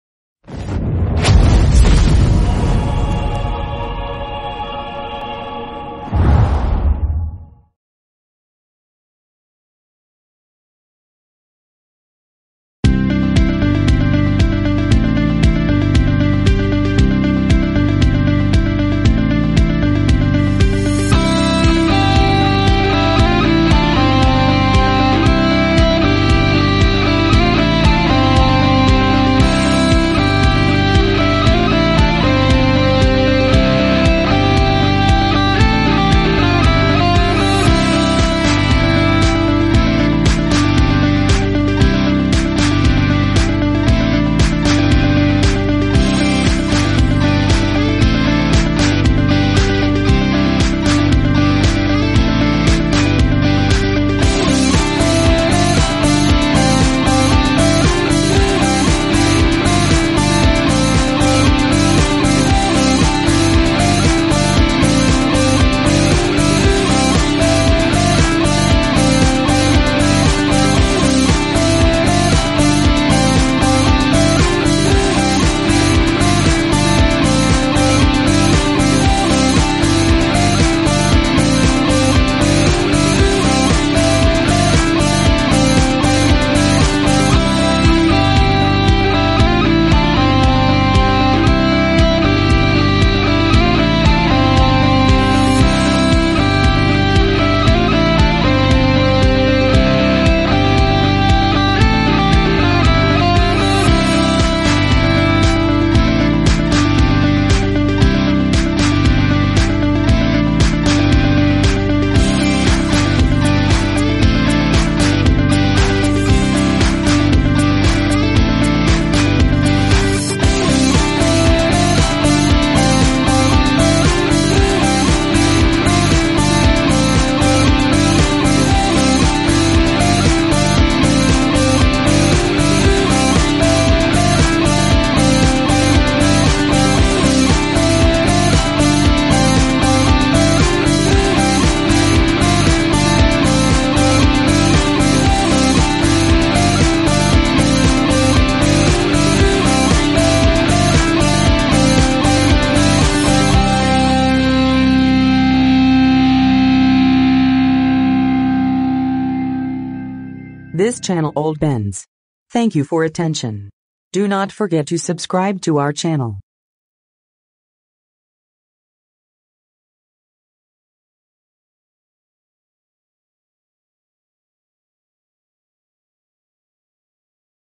NEW Mercedes-AMG E63 S Estate S212
Take your W212 generation Mercedes-AMG E63 S Estate to Fostla, give them approximately €9,000 ($10,320), and they will make it more powerful than the new one... . Video Title Mercedes-AMG E63 S Estate S212 by Fostla .